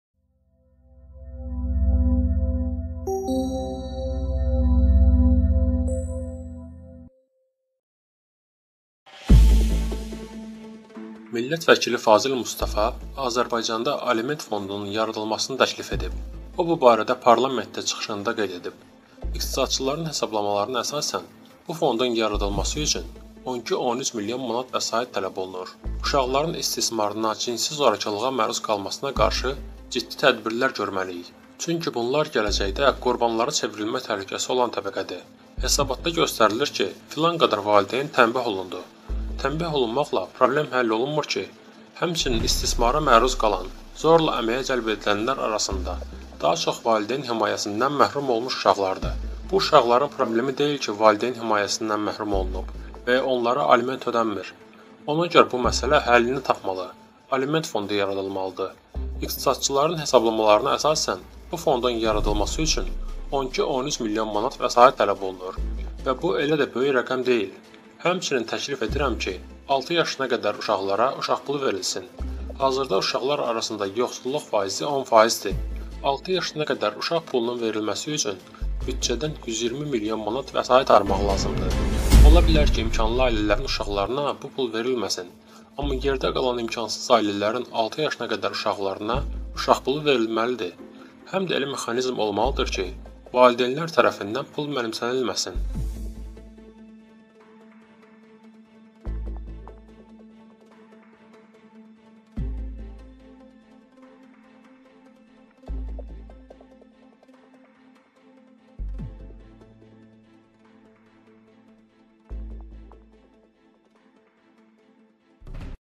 Fazil Mustafa:"Kasıb ailələrin uşaqlarına altı yaşa kimi “uşaq pulu” verilsin"
Millət vəkilli Fazil Mustafa Azərbaycanda Aliment Fondunun yaradılmasını təklif edib.

O, bu barədə parlamentdə çıxışında qeyd edib ki, iqtisadçıların hesablamalarına əsasən, bu fondun yaradılması üçün 12-13 milyon manat vəsait tələb olunur:

"Uşaqların istismarına, cinsi zorakılığa məruz qalmasına qarşı ciddi tədbirlər göməliyik. Çünki bunlar gələcəkdə qurbanlara çevrilmə təhlükəsi olan təbəqədir. Hesabatda (Milli Koordinatorun parlamentdəki hesabatını nəzərdə tutur - red.) göstərilir ki, filan qədər valideyn tənbeh olundu. Tənbeh olunmaqla, problem həll olunmur ki.

Həmçinin, istismara məruz qalan, zorla əməyə cəlb edilənlər arasında daha çox valideyn himayəsindən məhrum olunmuş uşaqlardır. Bu uşaqların problemi deyil ki, valideyn himayəsindən məhrum olunub və ya onlara aliment ödəmir. Ona görə bu məsələ həllini tapmalı, Aliment Fondu yaradılmalıdır.

İqtisadçıların hesablamalarına əsasən də bu fondun yaradılması üçün 12-13 milyon manat vəsait tələb olunur və bu, elə də böyük rəqəm deyil.

Həmçinin, təklif edirəm ki, altı yaşına qədər uşaqlara "uşaq pulu” verilsin. Hazırda uşaqlar arasında yoxsulluq faizi 10 faizdir. Altı yaşına qədər uşaq pulunun verilməsi üçün büdcədən 120 milyon manat vəsait ayırmaq lazımdır.

Ola bilər ki, imkanlı ailələrin uşaqlarına bu pul verilməsin. Amma yerdə qalan imkansız ailələrin altı yaşına qədər uşaqlarına "uşaq pulu” verilməlidir. Həm də elə mexanizm olmalıdır ki, valideynlər tərəfindən pul mənimsənilməsin”.